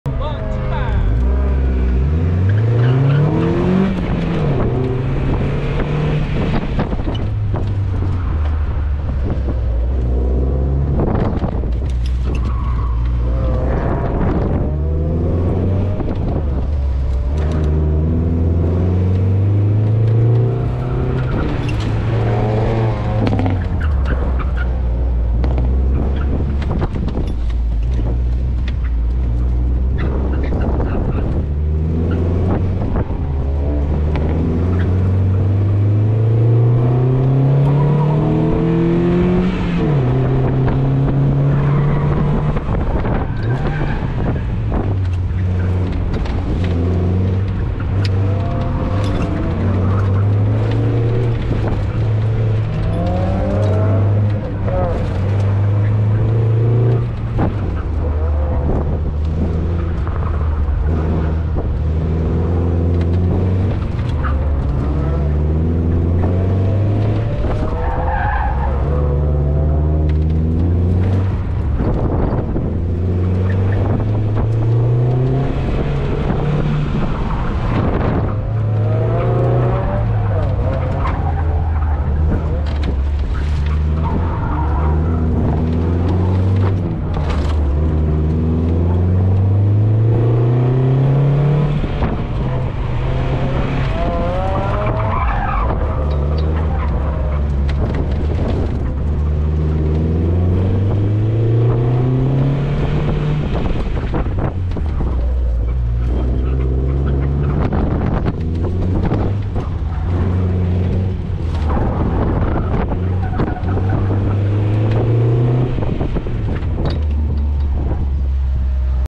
기아 엘란 짐카나 & 오토크로스 : 2023 레드콘 드라이빙 익스피리언스 1R
2023 레드콘 드라이빙 익스피리언스에 촬영겸 주행도 하러
놀러 다녀왔습니다.
촬영과 짐카나 참가에 의의를 두고 주행은 rpm은 낮게 , 코너는 빡시게
나름 열심히 주행했습니다.ㅋㅋ

영상은 인스타360 앞쪽 촬영한 3번째 연습주행 영상부터 업로드 !
재밌게 잘 즐기고 왔습니다.